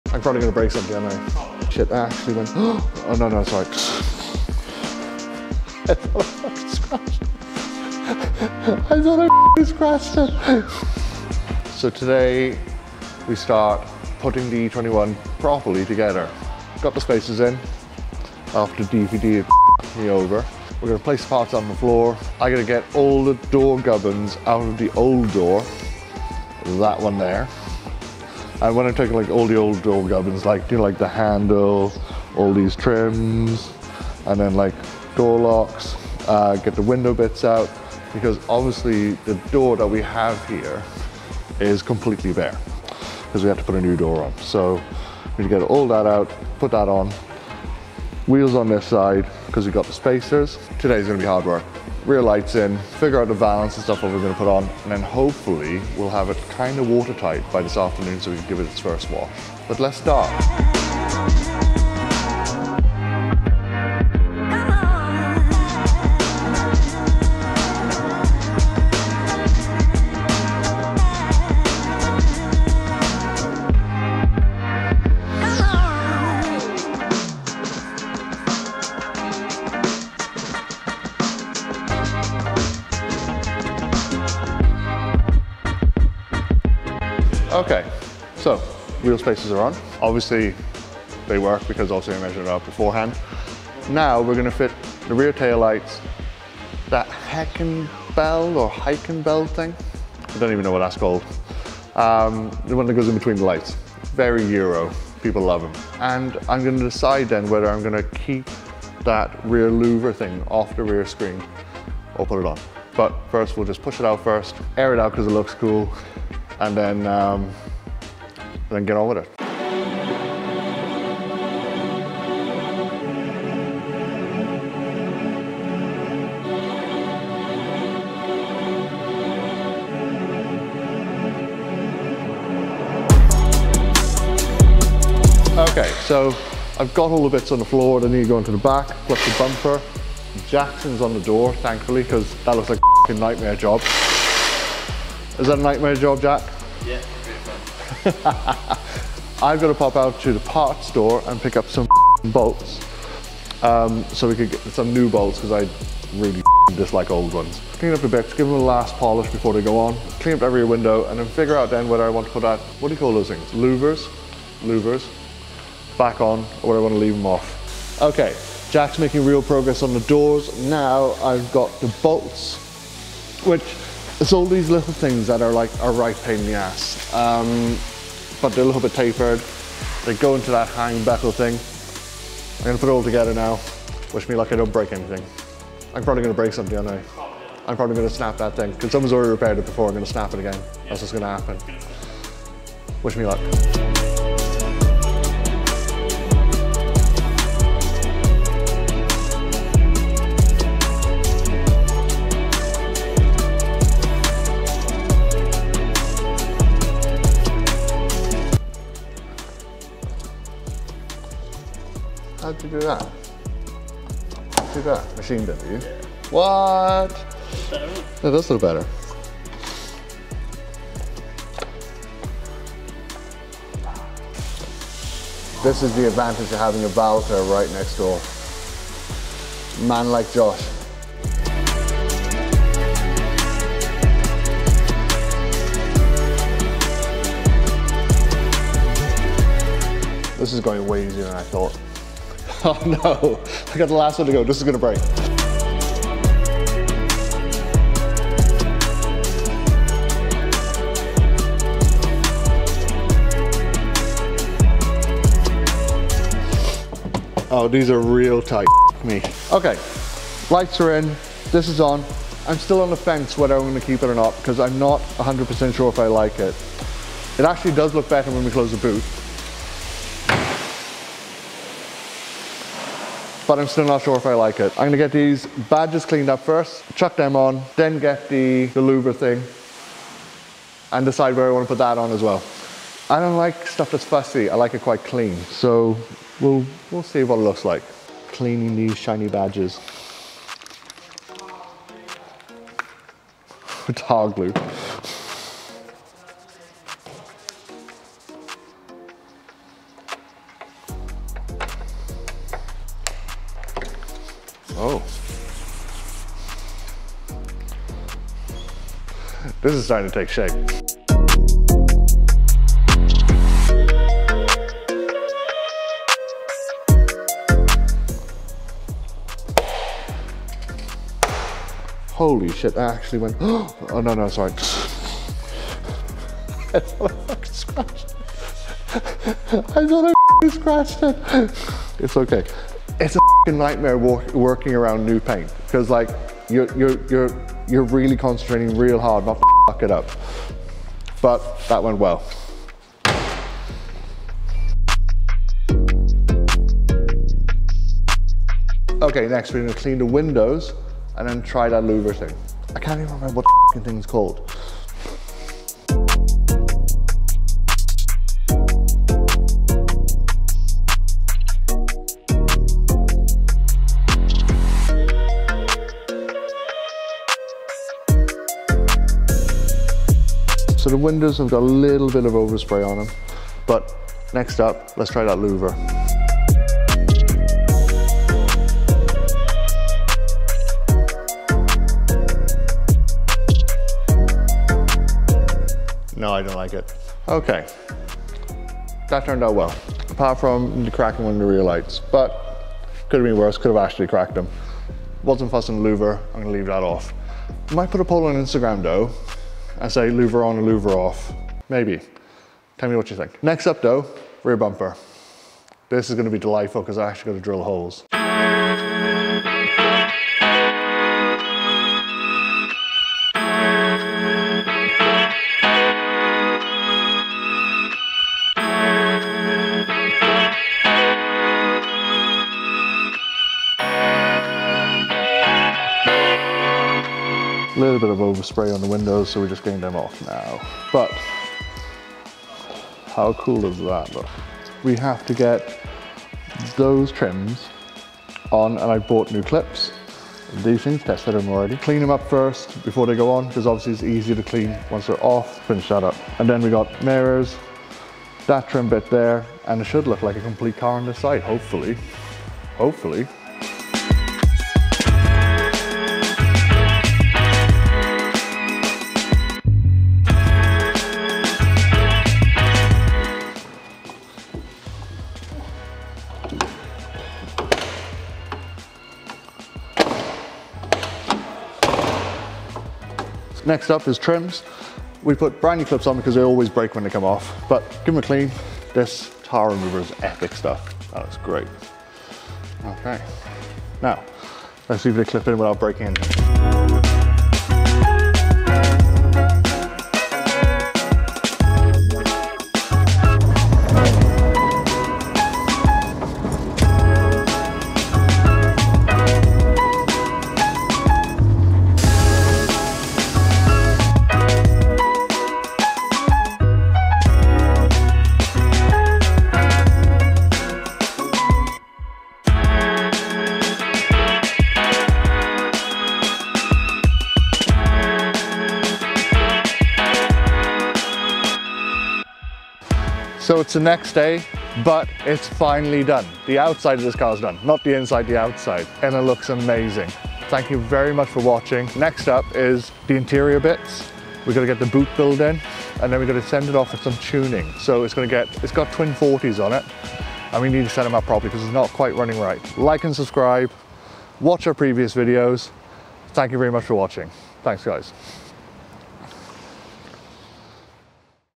BMW E21 BUILD UP
Today we build up the E21 project, turned out better than we expected!

@thebarnclassics on Instagram

The Barn Classic Cars on Facebook